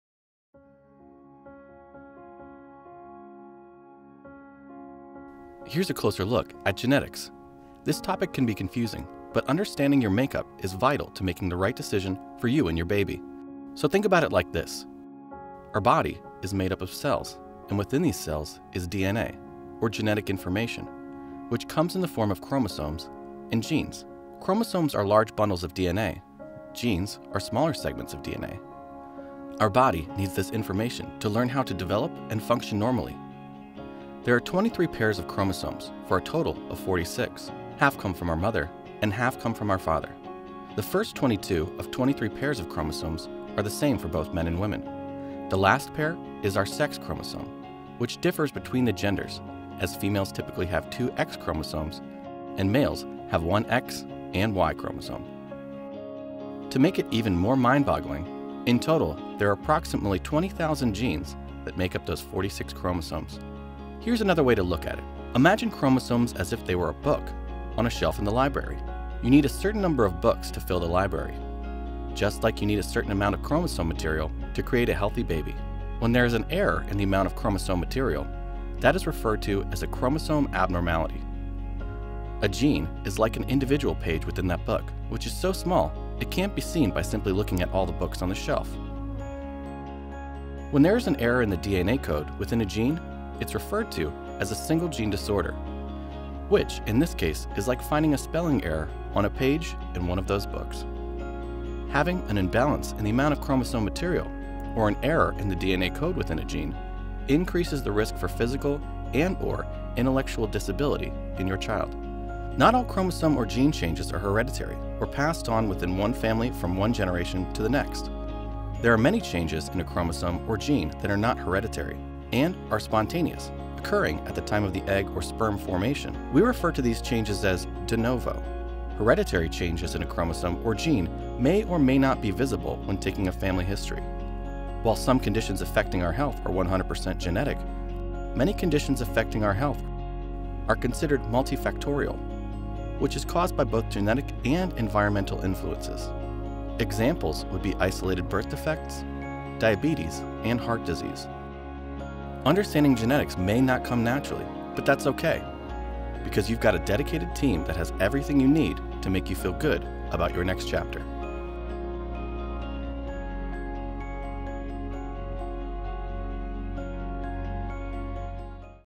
Understanding prenatal genetic testing
See how chromosomes and genes play a starring role in determining who we are.